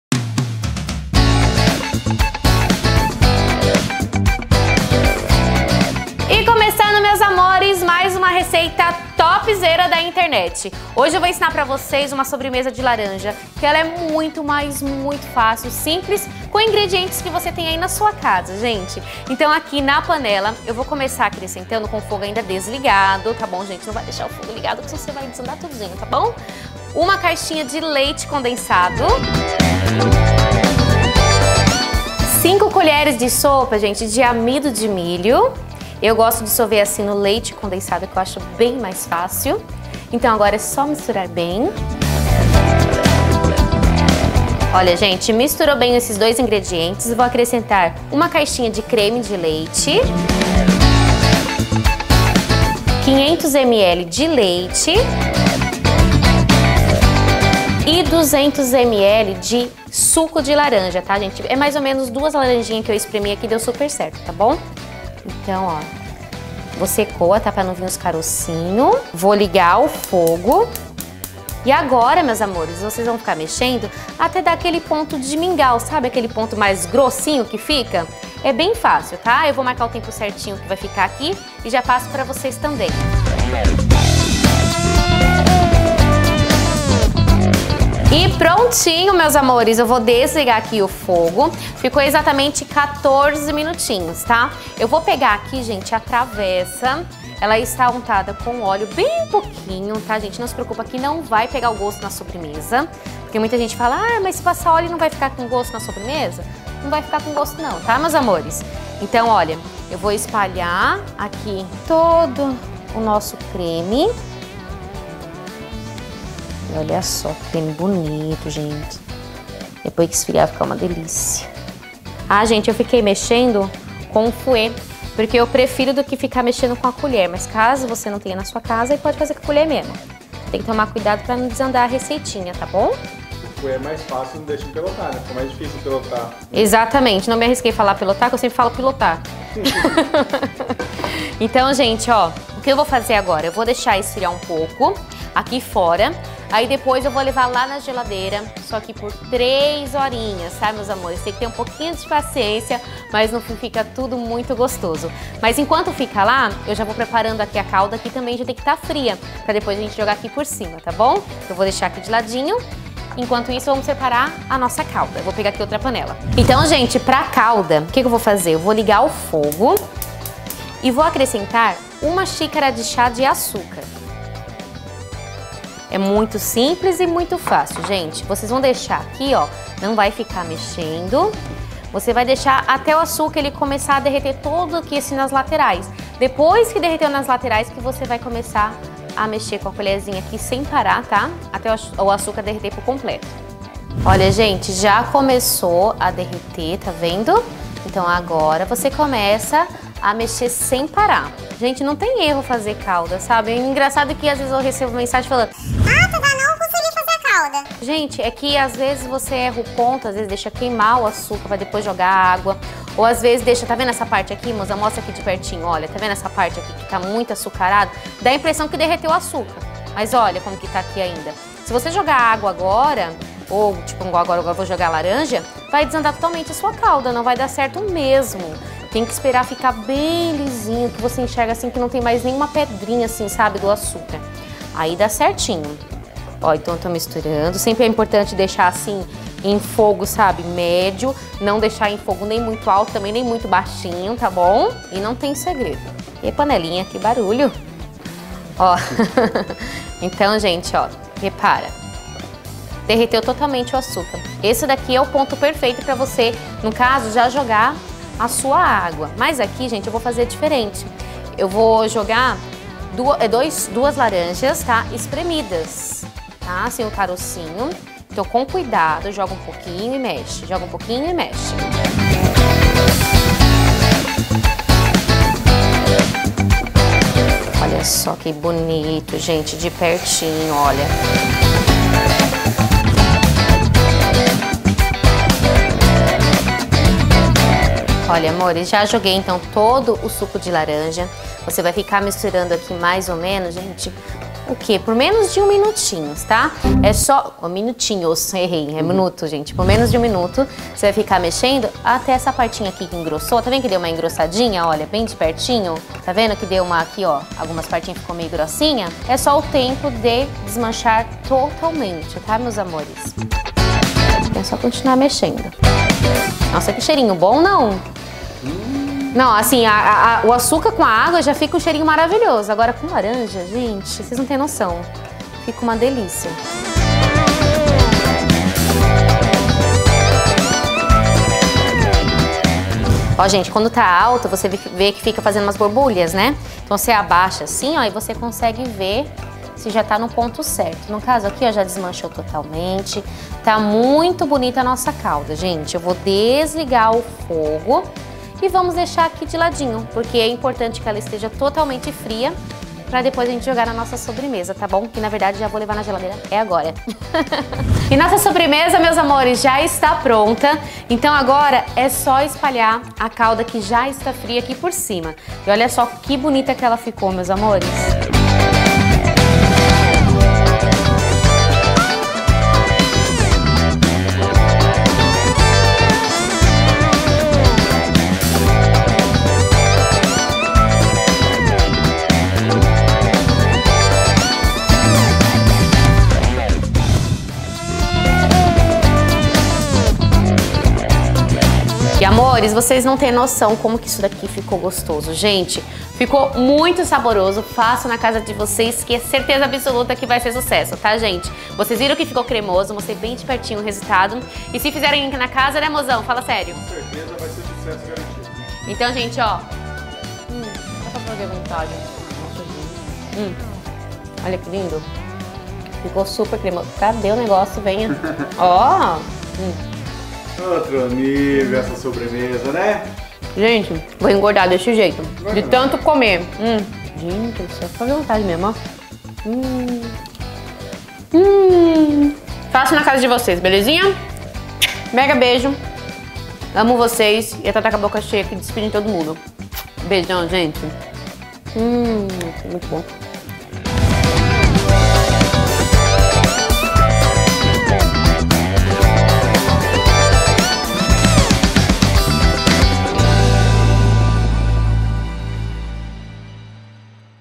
PUDIM DE LARANJA DE TRAVESSA EM CASA TODO MUNDO AMA ESSA SOBREMESA
Uma sobremesa fácil e deliciosa, bora surpreender a todos com essa receita, anote os ingredientes e bora começar!!!

VÍDEOS NOVOS TERÇA, QUINTA E SABADO 09:00

Os ingredientes são:
1 caixinha de leite condensado
5 colheres sopa de amido de milho
1 caixinha de creme de leite
500ml de leite
200ml de suco de laranja
Calda:
1 xícara (chá) de açúcar
2 laranjas espremidas

♡Para envios de correspondências e entregas:
Tata Pereira
Caixa Postal:1707
Sorocaba SP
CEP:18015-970